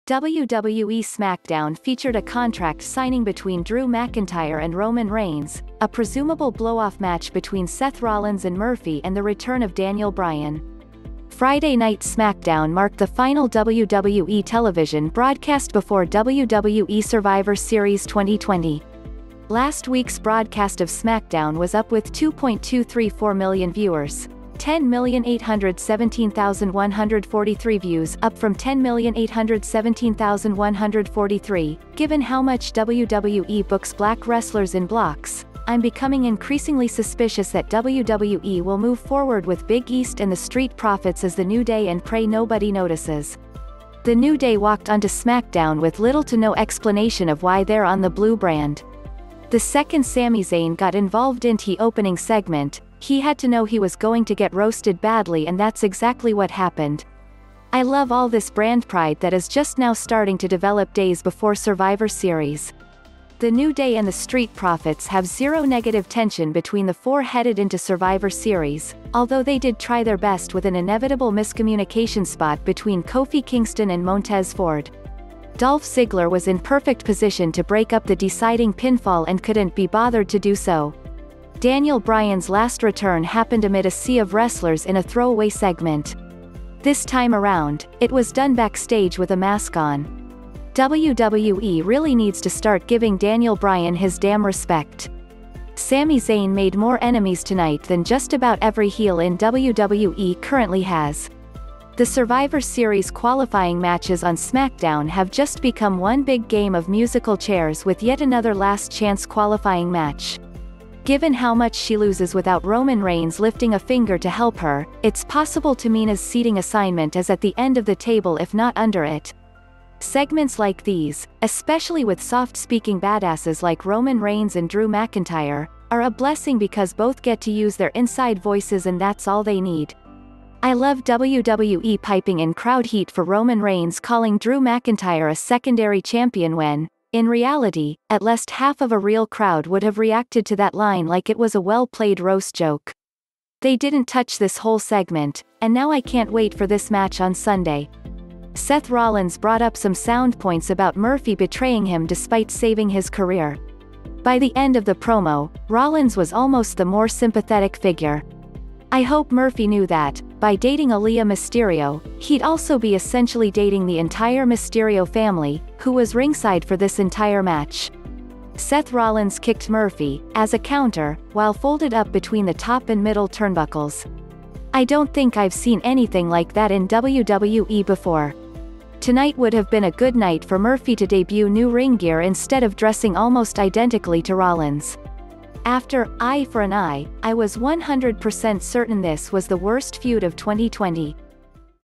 WWE SmackDown Results Winners News And Notes On November 20 2020
WWE SmackDown featured a contract signing between Drew McIntyre and Roman Reigns, a presumable blowoff match between Seth Rollins and Murphy and the return of Daniel Bryan. Friday Night SmackDown marked the final WWE television broadcast before WWE Survivor Series 2020. Last week’s broadcast of SmackDown was up with 2.234 million viewers.10,817,143 views (Up from 10,817,143)Given how much WWE books black wrestlers in blocks, I'm becoming increasingly suspicious that WWE will move forward with Big E and The Street Profits as The New Day and pray nobody notices. The New Day walked onto SmackDown with little to no explanation of why they're on the Blue Brand. The second Sami Zayn got involved int he opening segment, he had to know he was going to get roasted badly and that's exactly what happened. I love all this brand pride that is just now starting to develop days before Survivor Series. The New Day and The Street Profits have zero negative tension between the four headed into Survivor Series, although they did try their best with an inevitable miscommunication spot between Kofi Kingston and Montez Ford. Dolph Ziggler was in perfect position to break up the deciding pinfall and couldn't be bothered to do so. Daniel Bryan’s last return happened amid a sea of wrestlers in a throwaway segment. This time around, it was done backstage with a mask on. WWE really needs to start giving Daniel Bryan his damn respect. Sami Zayn made more enemies tonight than just about every heel in WWE currently has. The Survivor Series qualifying matches on SmackDown have just become one big game of Musical Chairs with yet another Last-Chance Qualifying Match. Given how much she loses without Roman Reigns lifting a finger to help her, it’s possible Tamina’s seating assignment is at the end of the table if not under it. Segments like these, especially with soft-speaking badasses like Roman Reigns and Drew McIntyre, are a blessing because both get to use their inside voices and that’s all they need. I love WWE piping in crowd heat for Roman Reigns calling Drew McIntyre a secondary champion when, in reality, at lest half of a real crowd would have reacted to that line like it was a well-played roast joke. They didn’t touch this whole segment, and now I can’t wait for this match on Sunday. Seth Rollins brought up some sound points about Murphy betraying him despite saving his career. By the end of the promo, Rollins was almost the more sympathetic figure. I hope Murphy knew that, by dating Aalyah Mysterio, he’d also be essentially dating the entire Mysterio family, who was ringside for this entire match. Seth Rollins kicked Murphy, as a counter, while folded up between the top and middle turnbuckles. I don’t think I’ve seen anything like that in WWE before. Tonight would have been a good night for Murphy to debut new ring gear instead of dressing almost identically to Rollins. After "Eye for an Eye," I was 100% certain this was the worst feud of 2020.